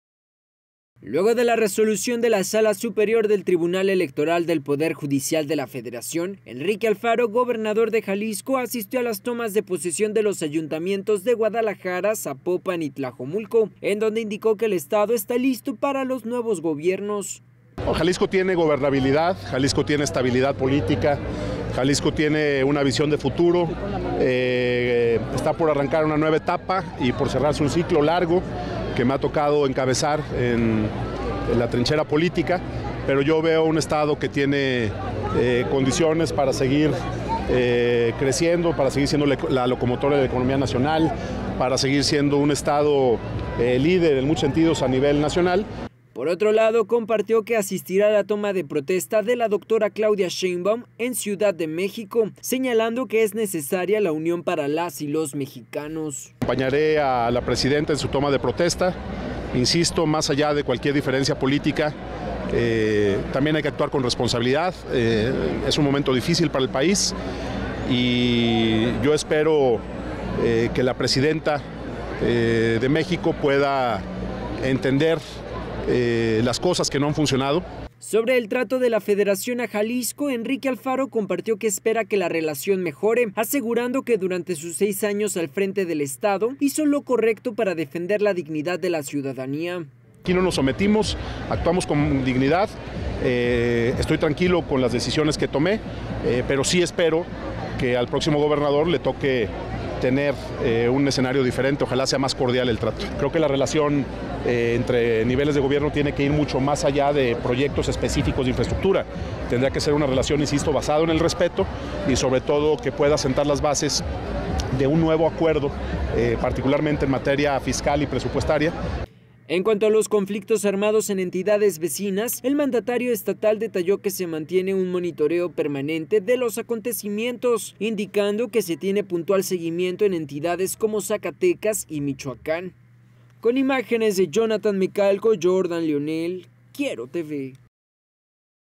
Enrique Alfaro informa sobre las transiciones de gobierno
🗣️⏭️Luego de la toma de protesta de Juan José Frangie, presidente municipal de Zapopan, el gobernador de Jalisco, Enrique Alfaro Ramírez, habló sobre las transiciones de gobierno y la relación de la federación con el estado.